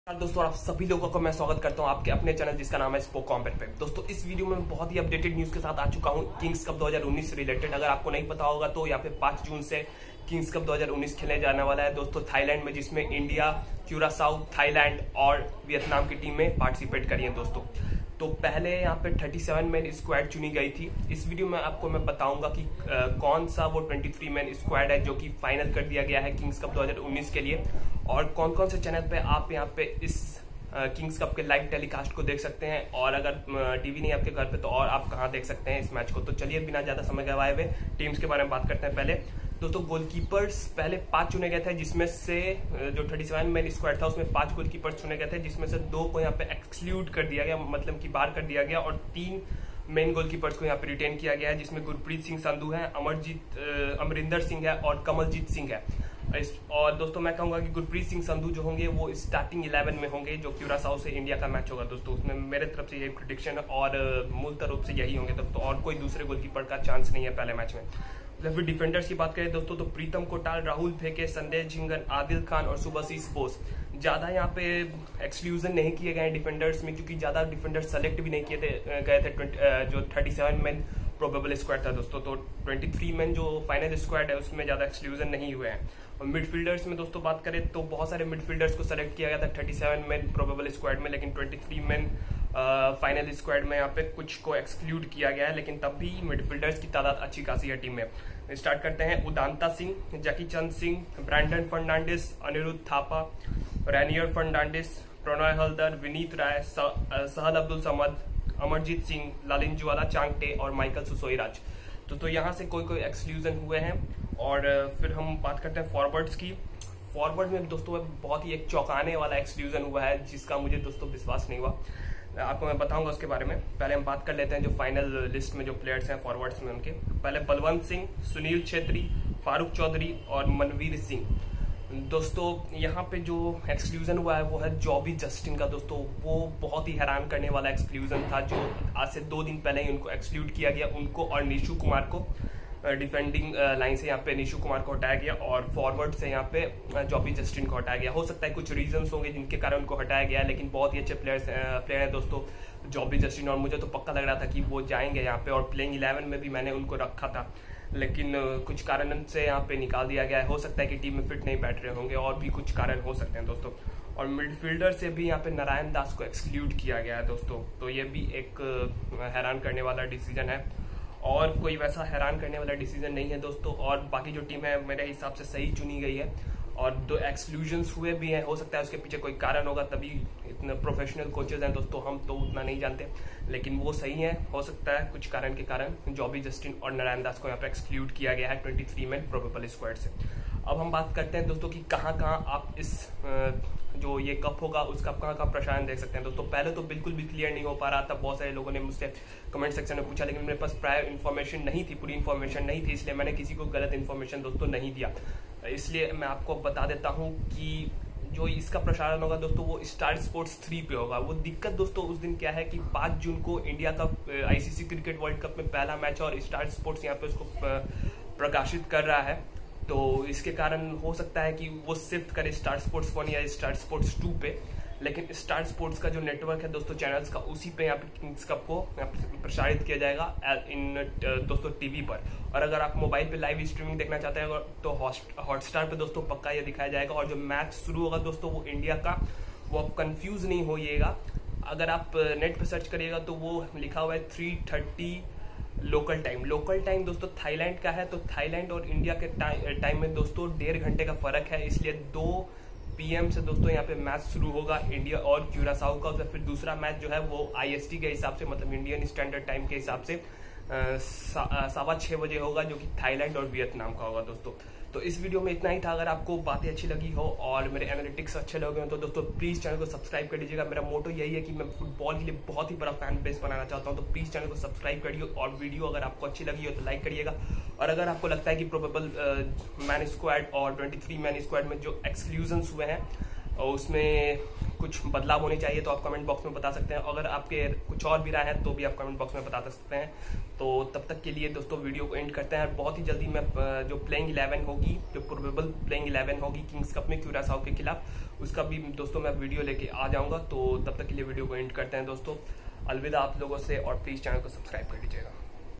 Kings Cup 2019|Where to watch?|23 men squad|Full scedule
This video is about Kings Cup 2019. This will be played in Thailand. 23 men Indian Squad. Full schedule. Where to watch.

23 men full squad
Goalkeepers: Gurpreet Singh Sandhu, Amrinder Singh, Kamaljit Singh

Defenders: Pritam Kotal, Rahul Bheke, Sandesh Jhingan, Adil Khan, Subhasish Bose

Midfielders: Udanta Singh, Jackichand Singh, Brandon Fernandes, Anirudh Thapa, Raynier Fernandes, Pronay Halder, Vinit Rai, Sahal Abdul, Amarjit Singh, Lallianzuala Chhangte, Michael Soosairaj

Forwards: Balwant Singh, Sunil Chhetri, Farukh Choudhary, Manvir Singh

Where to watch:
Stars Sports 3
Hotstar
Timing:-
2 pm
5th June